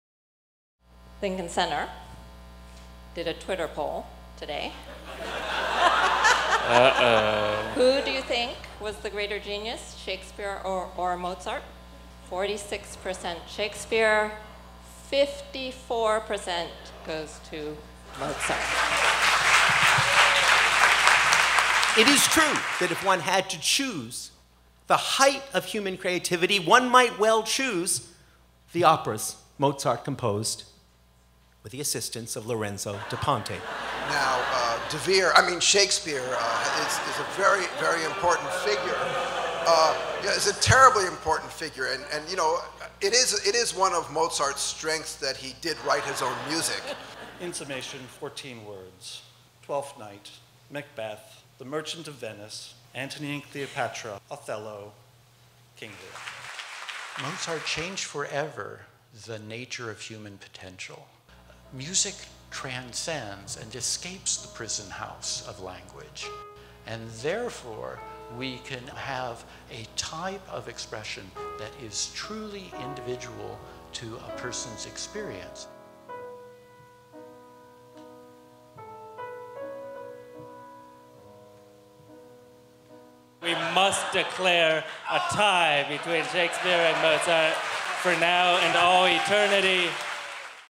Shakespeare vs. Mozart: A Library Debate
Who is the greater genius, Shakespeare or Mozart? Experts debated the question on April 1, 2016, at the New York Public Library for the Performing Arts as a prelude to the 50th Mostly Mozart Festival.